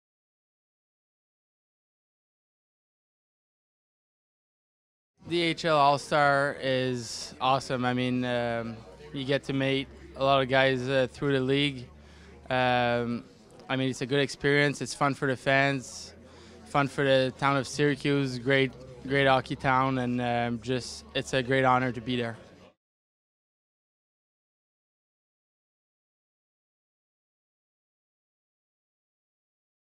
2016 Toyota AHL All-Star Classic Jonathan Marchessault
2015 AHL All Star Jonathan Marchessault talks about his experience in the AHL All-Star Classic. Tickets for the 2016 Toyota AHL All-Star Classic are on sale now!